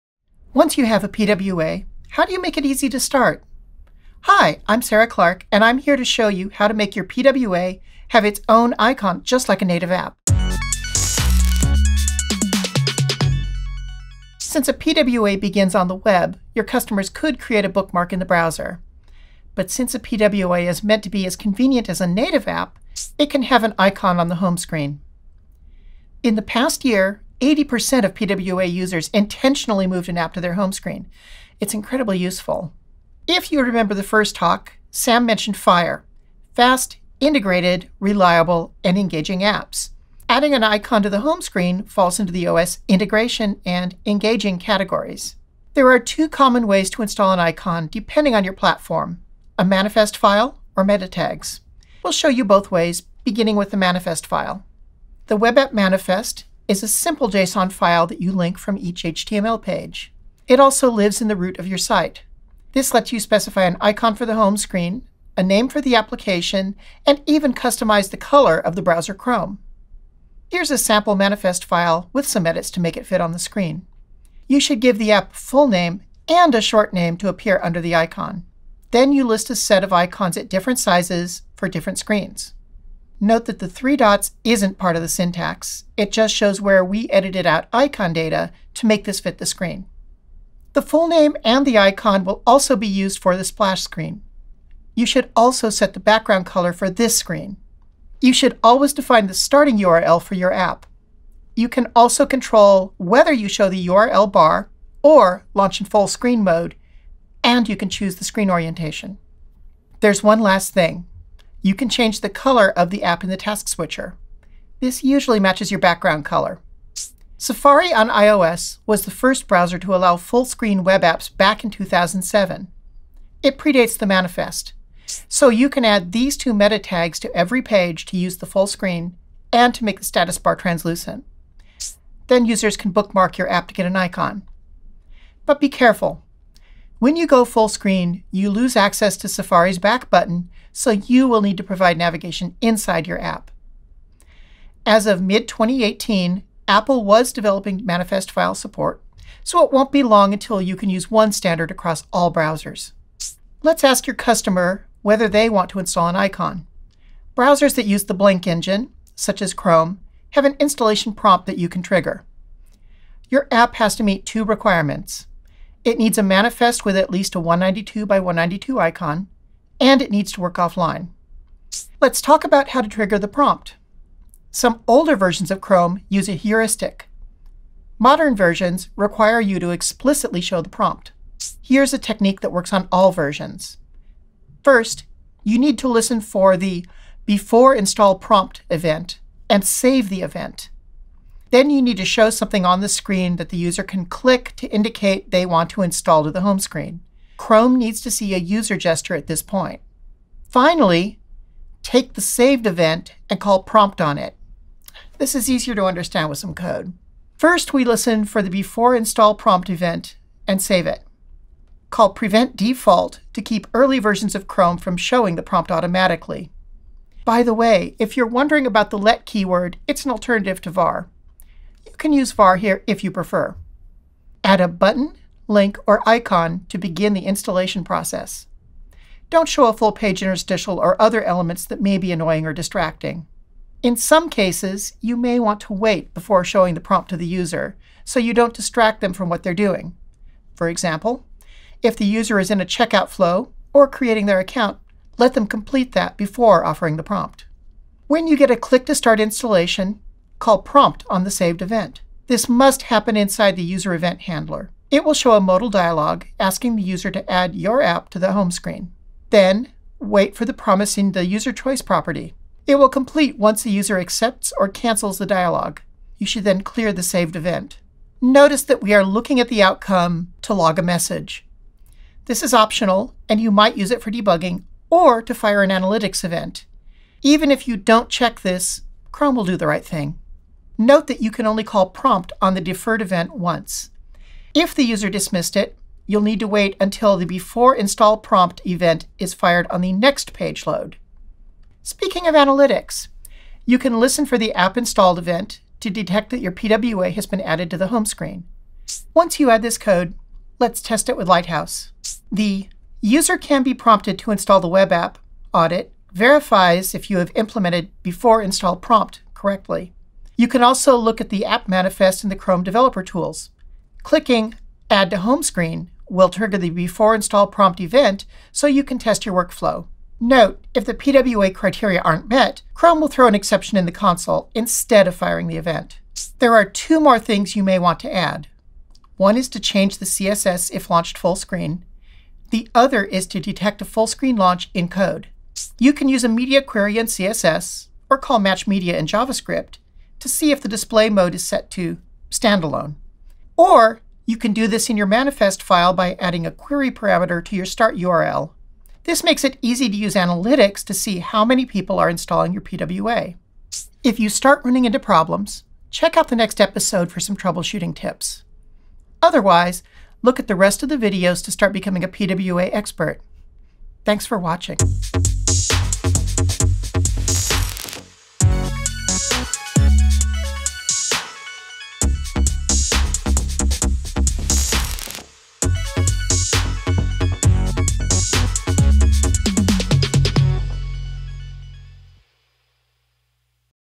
Install a PWA on the home screen - Progressive Web App Training
Your PWA can have a presence on the user's device — just like a native app.

This video shows you how.